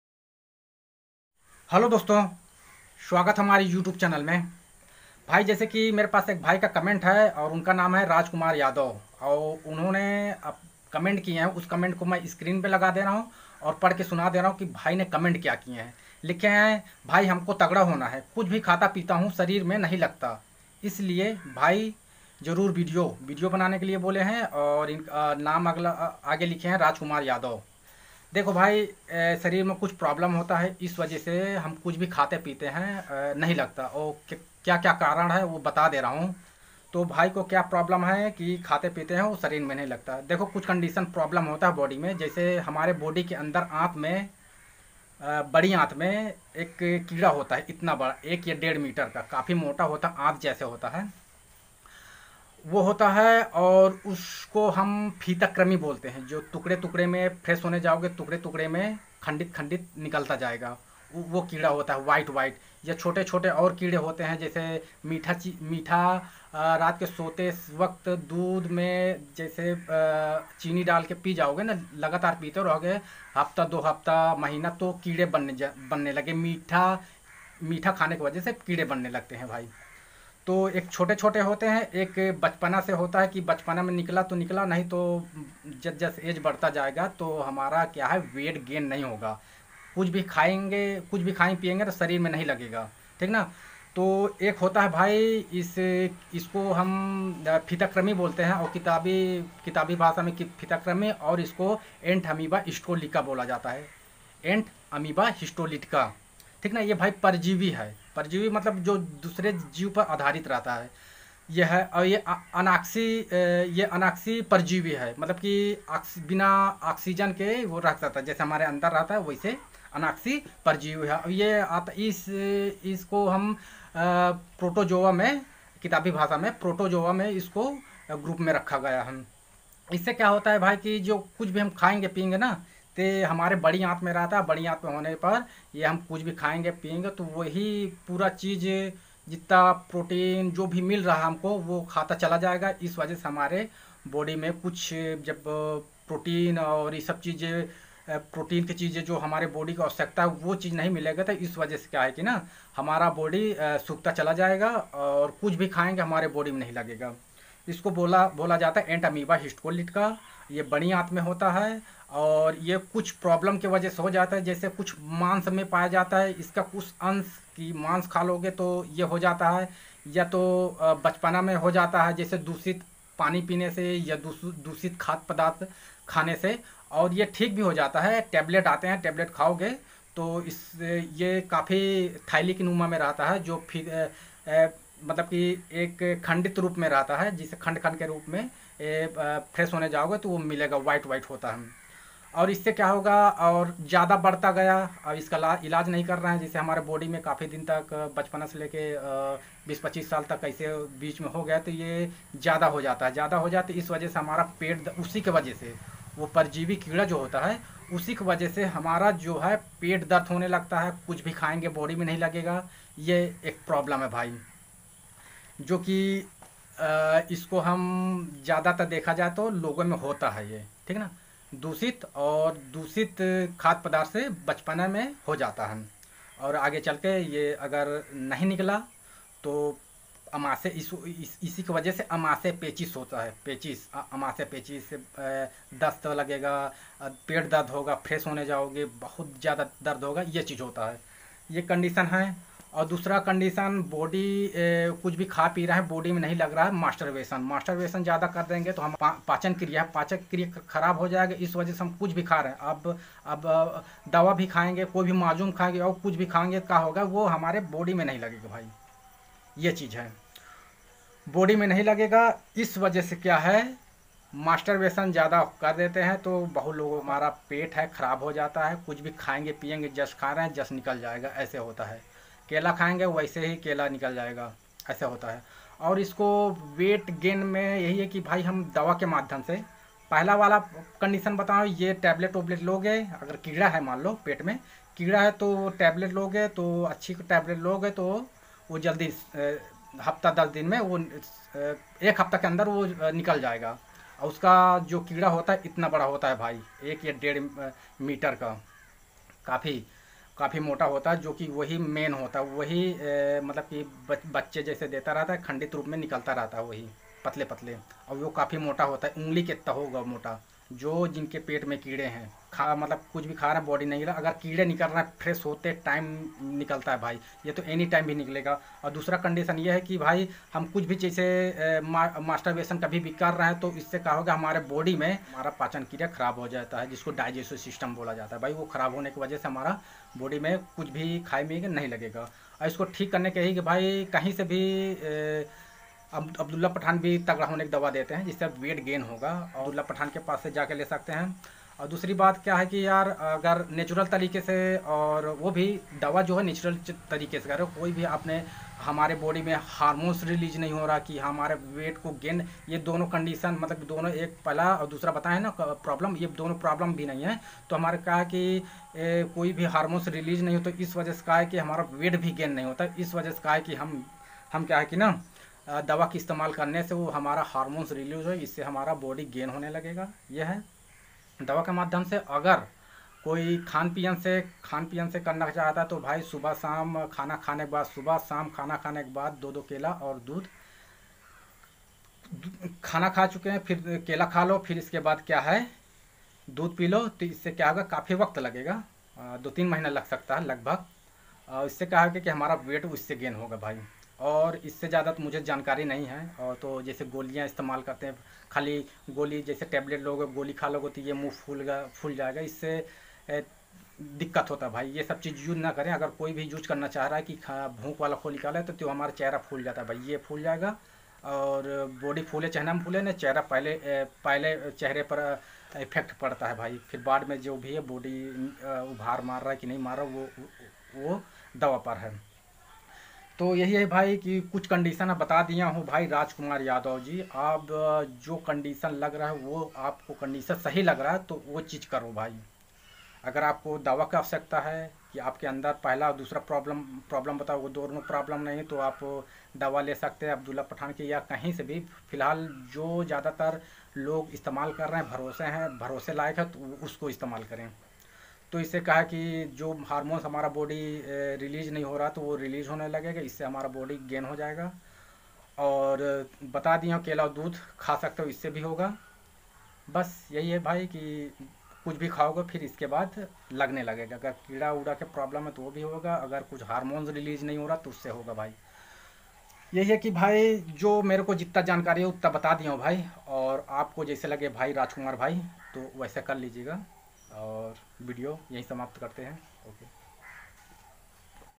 Health kyo nahi ban rha hai jane
Description:-

Hello friends aaj ki is video me mai Rajkumar yadav ka health n banne ka karan batayenge joki aap logo ko pasand aye g .